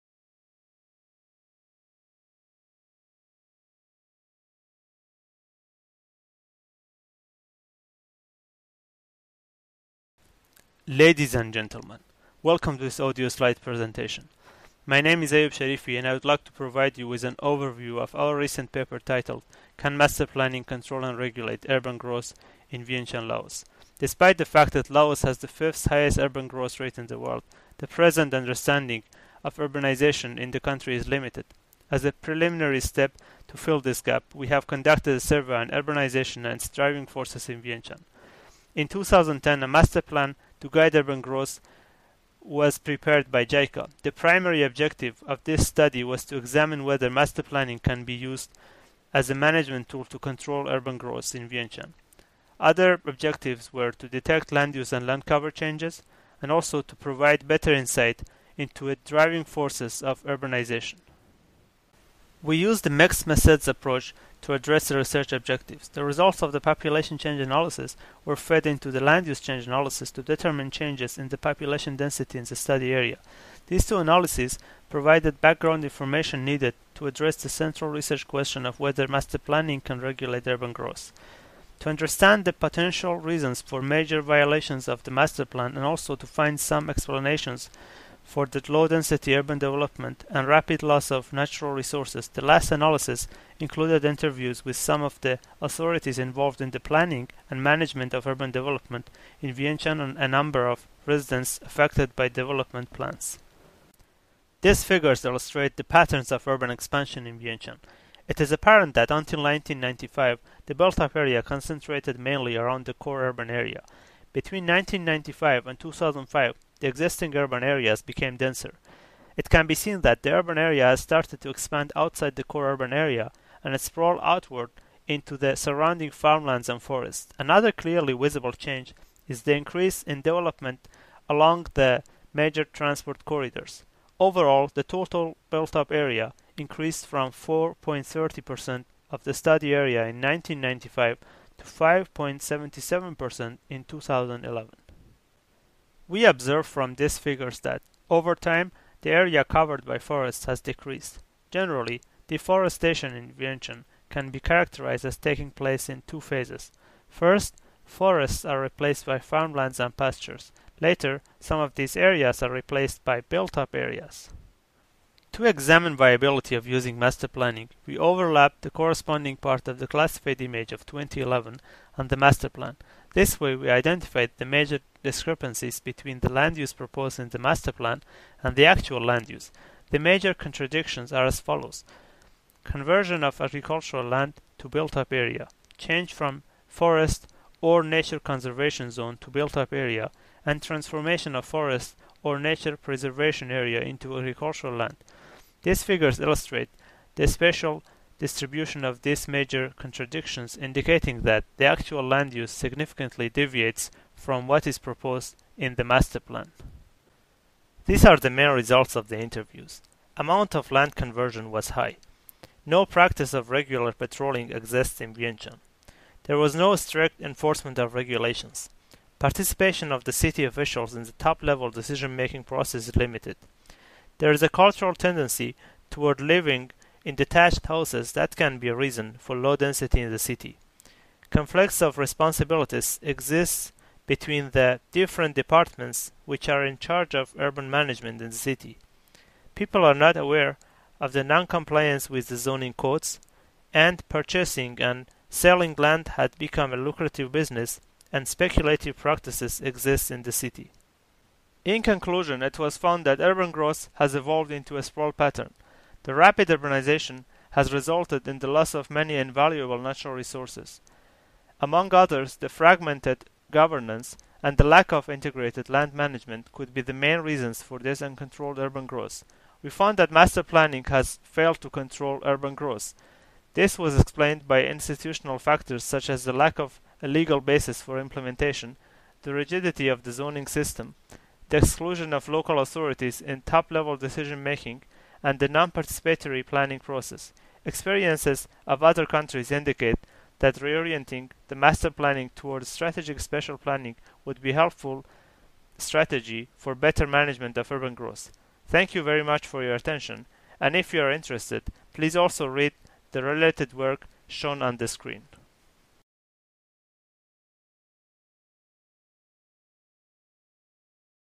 Can master planning control and regulate urban growth in Vientiane, Laos?
The rate of urbanization in Laos has increased significantly since the country began its transition to a market economy in 1986. Although urbanization in developing countries has been examined extensively in the literature, very few researchers have focused on Laos. Our main aim here was to examine the effectiveness of utilizing the master plan as a management tool to regulate urban growth in Vientiane, the capital city of Laos. We used a mixed-methods approach to address the research objective. First, we examined the population changes since 1985. Next, land-use and land-cover changes were detected by analyzing the satellite images from three different time periods. Then, we analyzed the city's recently approved master plan to determine whether it can regulate urban growth. Finally, to investigate the socio-economic and institutional issues and better understand the reasons for rapid land-use and land-cover changes, we interviewed several key figures from the departments related to urban management and a number of residents living in the peri-urban area. Between 1995 and 2011, the urban population in
the study area increased from 337,000 to 780,000. In the same time period, the built-up area increased from approximately 154 square kilometers to more than 206 square kilometers. The total area of the forests decreased from approximately 2,354 square kilometers to approximately 1,353 square kilometers. The results indicate that there is an urgent need for improvements in the regulatory and management aspects and that the master plan should be revised to provide a more realistic account of the existing situation.